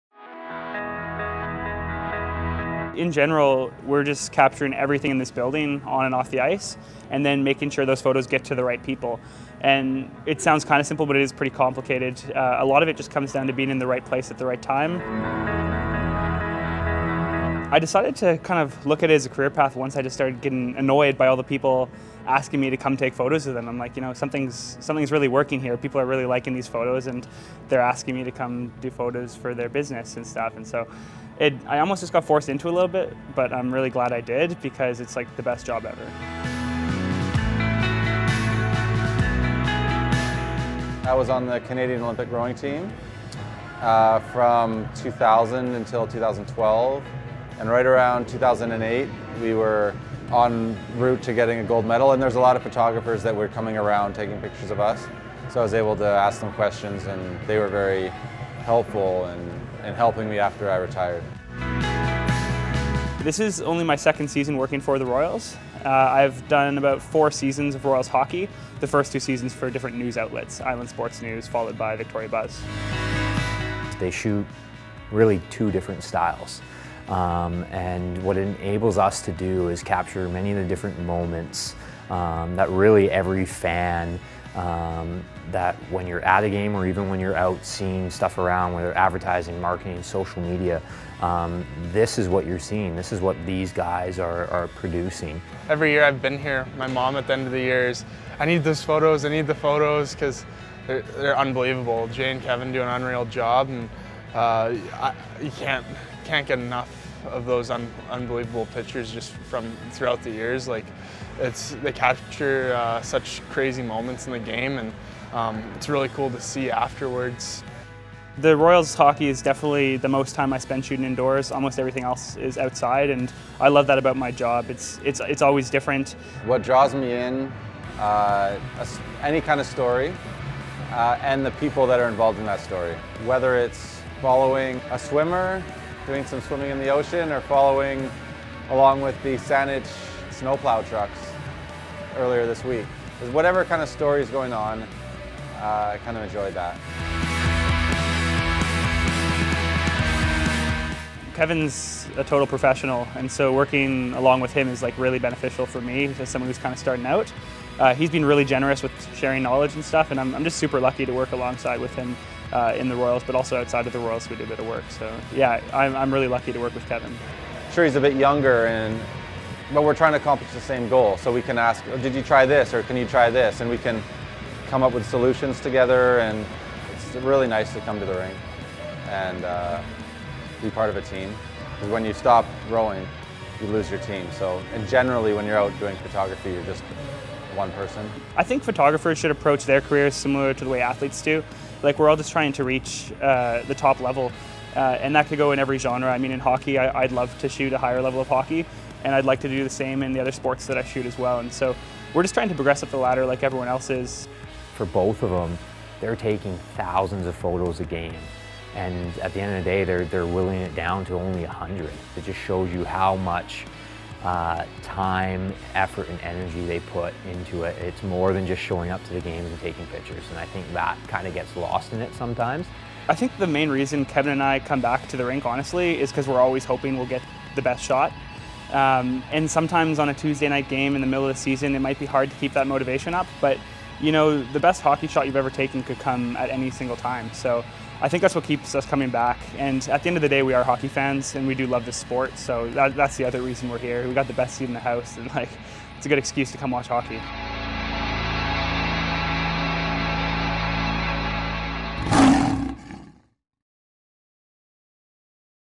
Royals TV - Picture Perfect
You see their photos on social media, the internet and in advertisements, now take a behind the scenes look how Royals' photographers Jay Wallce and Kevin Light go about their craft!